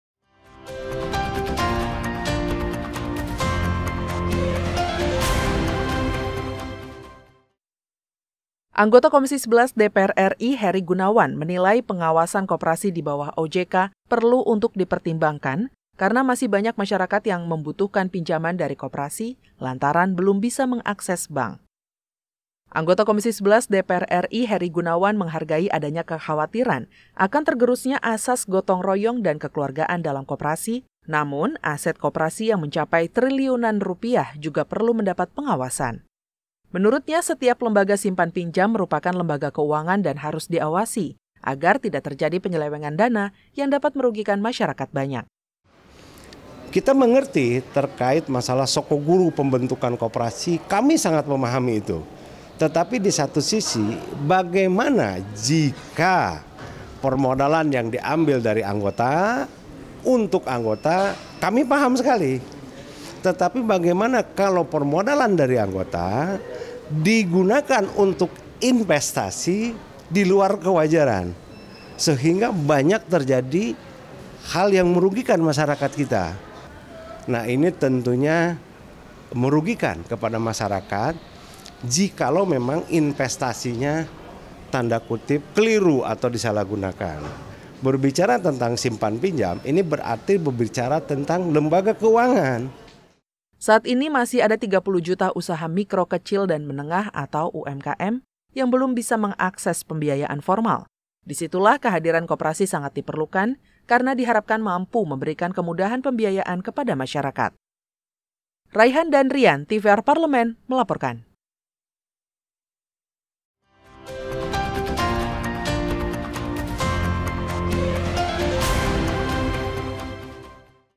TVR 120 - Komisi XI DPR RI: Pengawasan Koperasi Oleh OJK Dinilai Perlu
Anggota Komisi XI DPR RI Heri Gunawan menilai, pengawasan koperasi di bawah OJK perlu untuk dipertimbangkan, karena masih banyak masyarakat yang membutuhkan pinjaman dari koperasi lantaran belum bisa mengakses bank.

Narasumber:
Heri Gunawan / Anggota Komisi XI DPR RI / F – PGerindra / Jabar IV